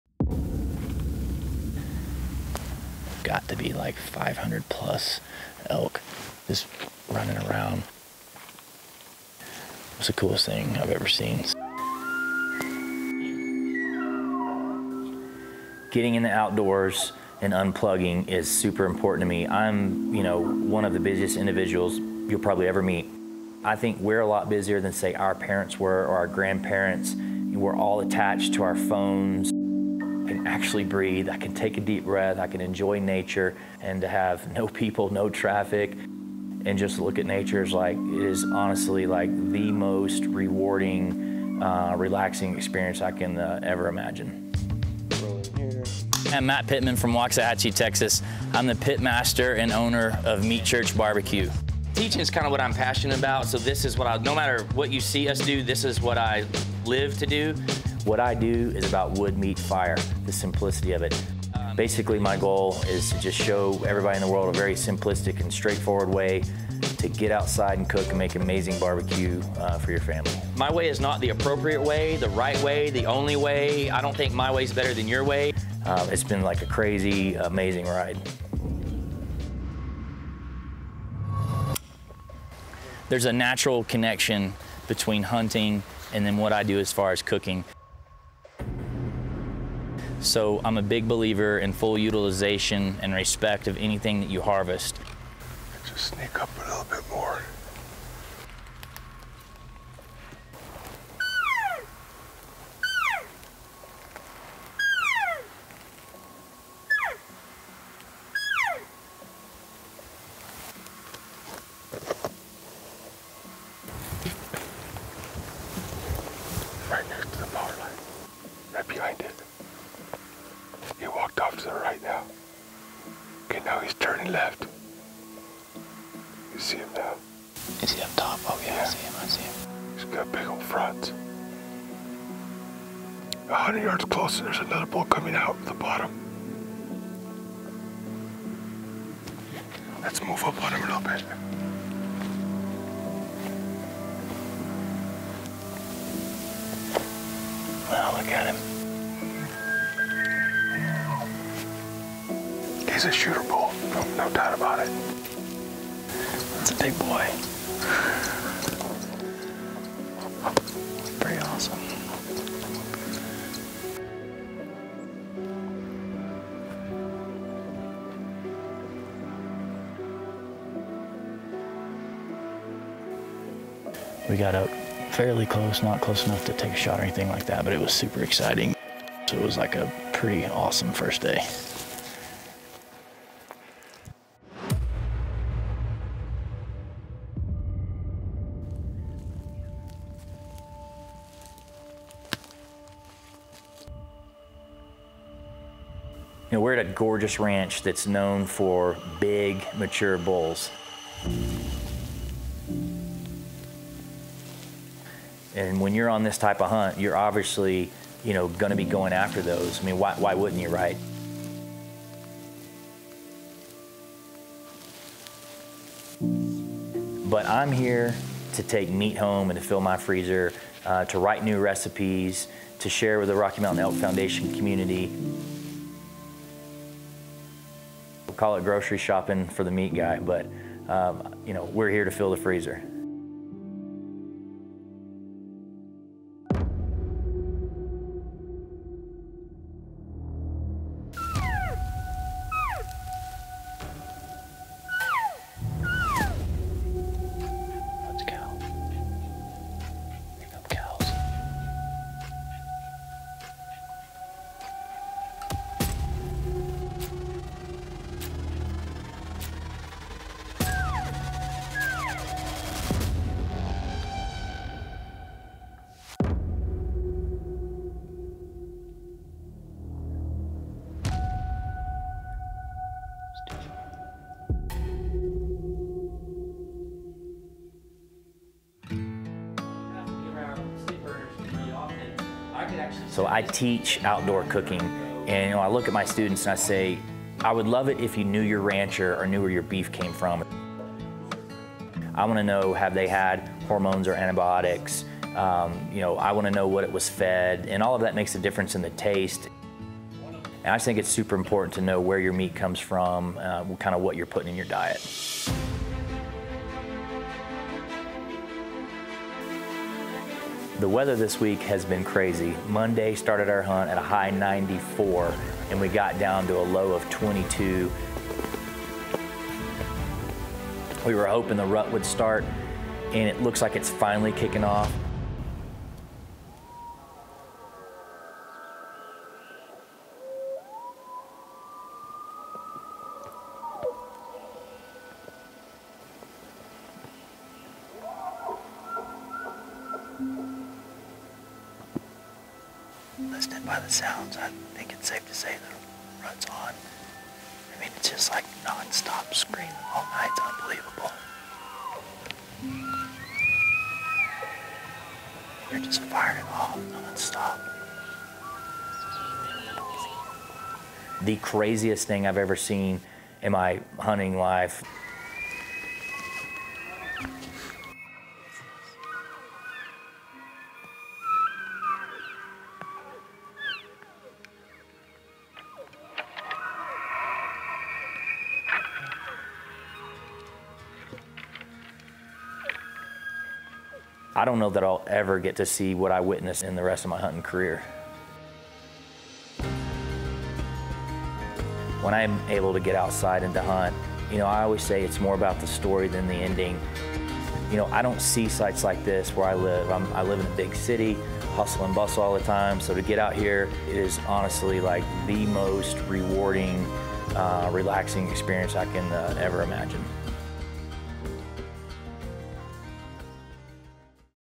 Meat Church's Matt Pittman Hunts UUBar - An RMEF Film
Matt Pittman of Meat Church BBQ steps away from the smoke pit and takes up his  bow and arrow for a northern New Mexico elk hunt.

"There's a natural connection between hunting and what I do as far as cooking. I'm a big believer in full utilization and respect of anything that you harvest."
- Matt Pittman